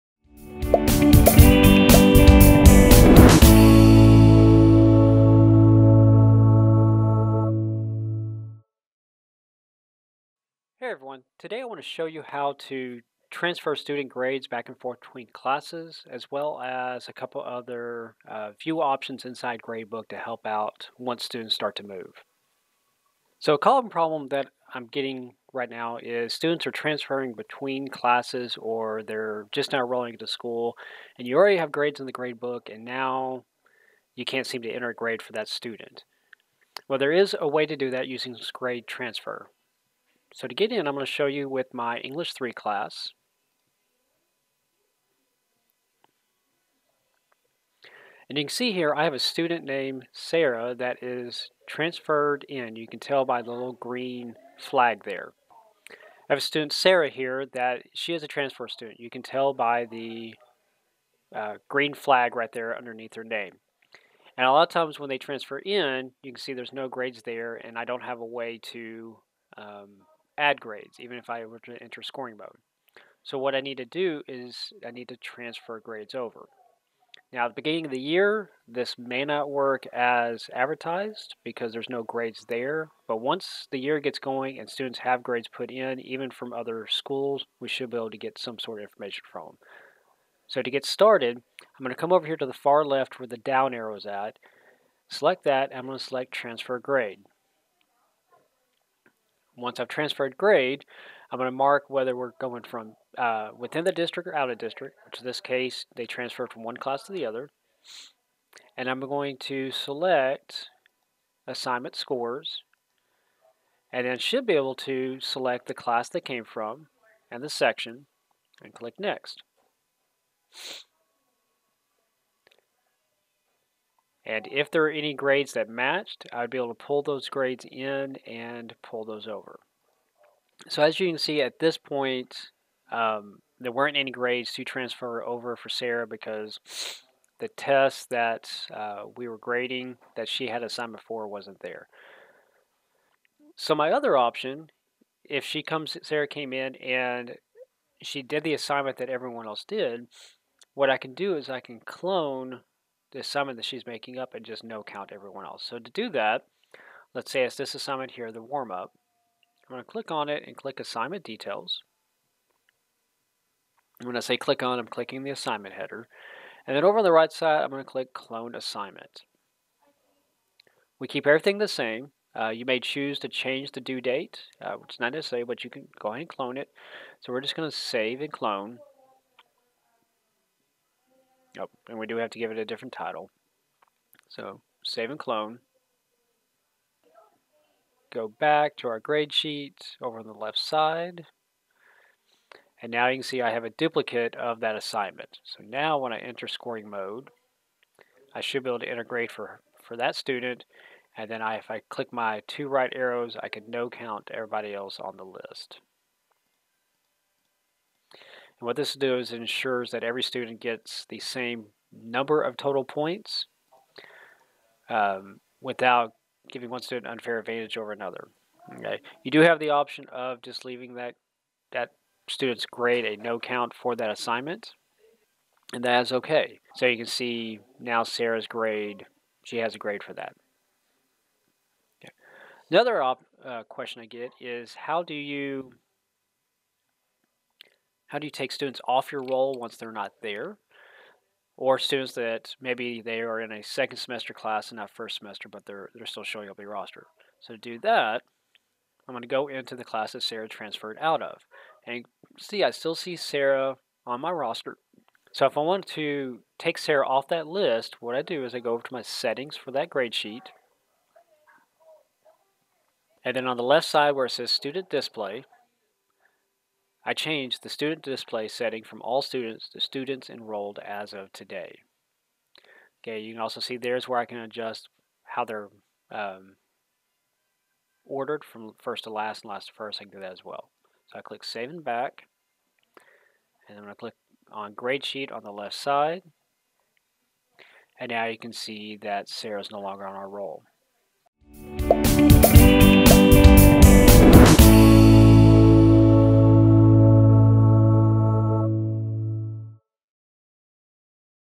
Qmlativ Dealing with Transfer Student Grades and Removing Students
This is a quick video on how to use the Transfer Grades feature in Qmlativ and how to change view options in order to remove past students from gradesheet.